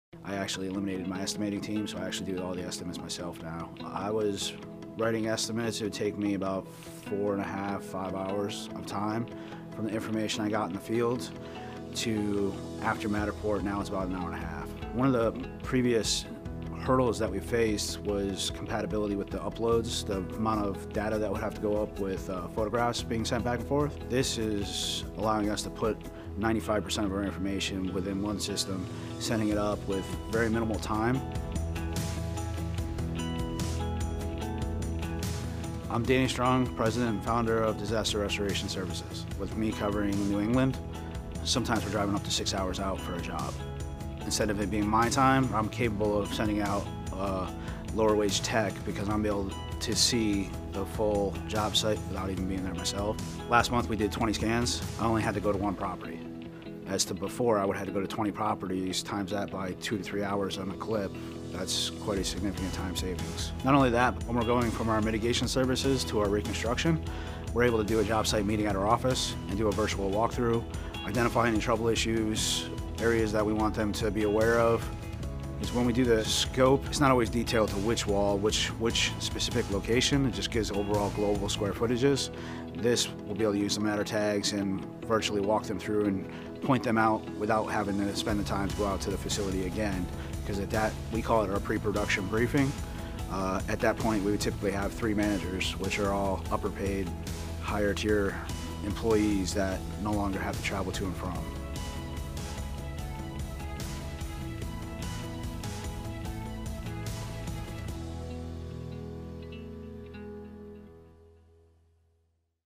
DRS embraces a 3D-led approach to help clients recover faster - A Matterport Customer Story
Recent advances in 3D technology and collaboration platforms challenged Disaster Restoration Services (DRS) to reimagine documentation, estimation, and insurance adjuster engagement to help customers restore properties faster.